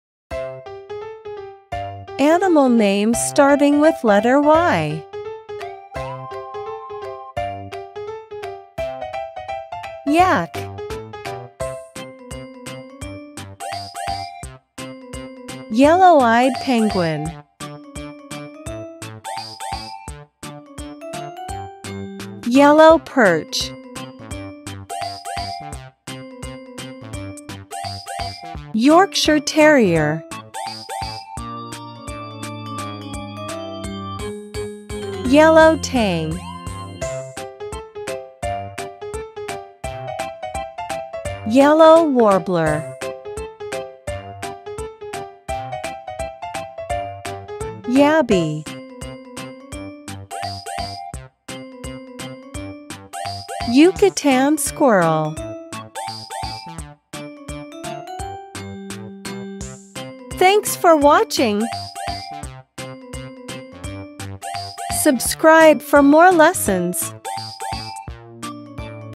Animals Starting with the Letter "Y" | English Vocabulary | Animals with Pictures for Kids
Join us on an exciting journey through the animal kingdom as we explore animals that start with the letter "Y"! This engaging video for kids features captivating pictures, clear pronunciations, and fun facts to make learning English vocabulary an enjoyable experience.